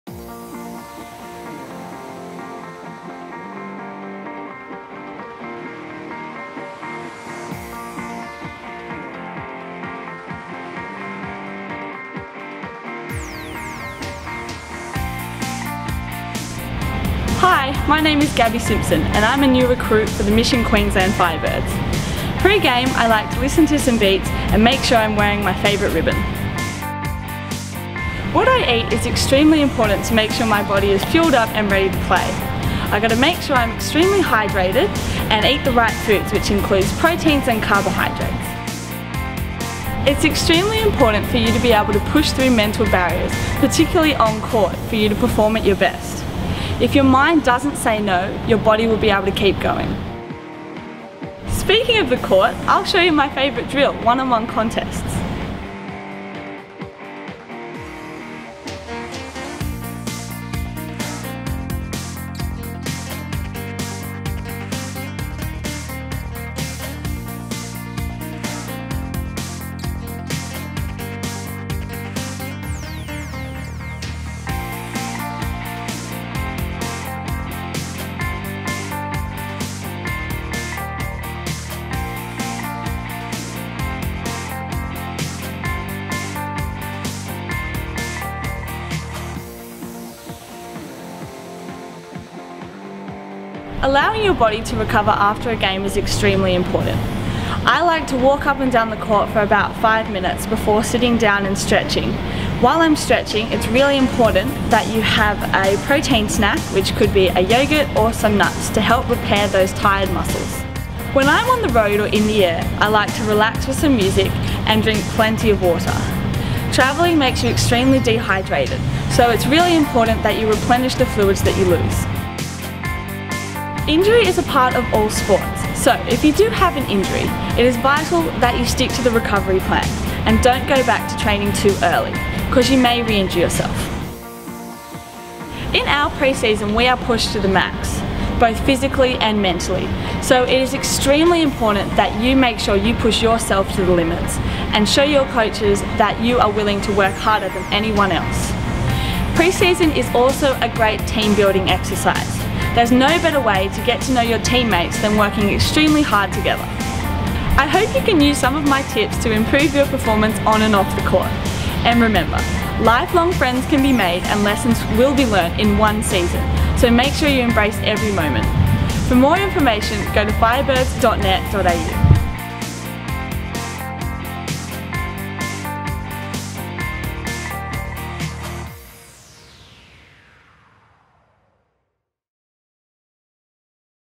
Firebirds Skills Video: Gabi Simpson
Watch as Gabi Simpson demonstrates 1 v 1 - Contest the First Ball.